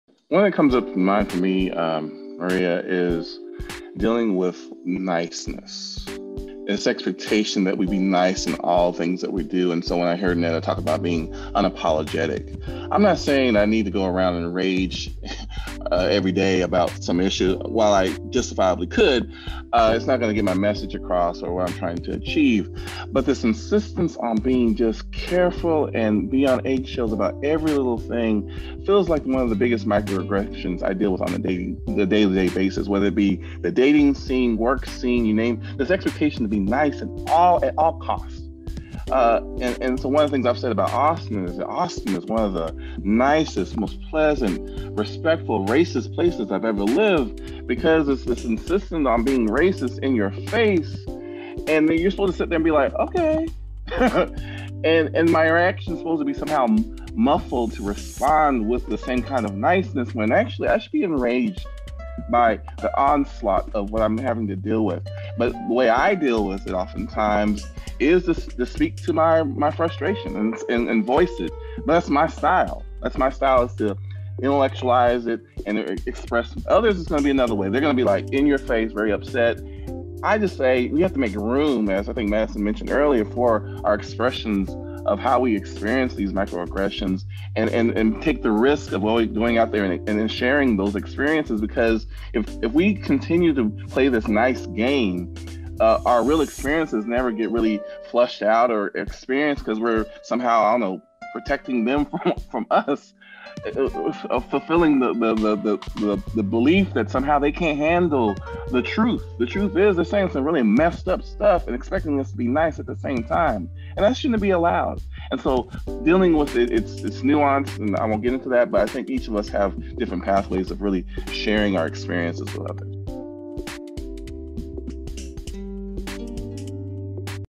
The Expectation of Nice
There's this expectation that we be nice in all things that we do.

I'm not saying I need to go around and rage every day about some issue.

But this insistence on being careful and walking on eggshells about every little thing feels like one of the biggest microaggressions I deal with on a day-to-day basis.

One of the things I've said about Austin is, "Austin is one of the nicest, most pleasant, respectful, racist places I've ever lived."

Because of its insistence on being racist in your face and then you're supposed to sit there and be like, "Okay."

And my reaction is supposed to be somehow muffled to respond with the same kind of niceness, when actually I should be enraged by the onslaught of what I'm having to deal with.

The way I deal with it oftentimes is to speak to my frustration and voice it.

Because if we continue to play this nice game, our real experiences never really get fleshed out or experienced because we're somehow protecting them from us, fulfilling the belief that somehow they can't handle the truth.

The truth is they're saying some really messed up stuff and expecting us to be nice at the same time, and that shouldn't be allowed.

I think each of us have different pathways of really sharing our experiences with others.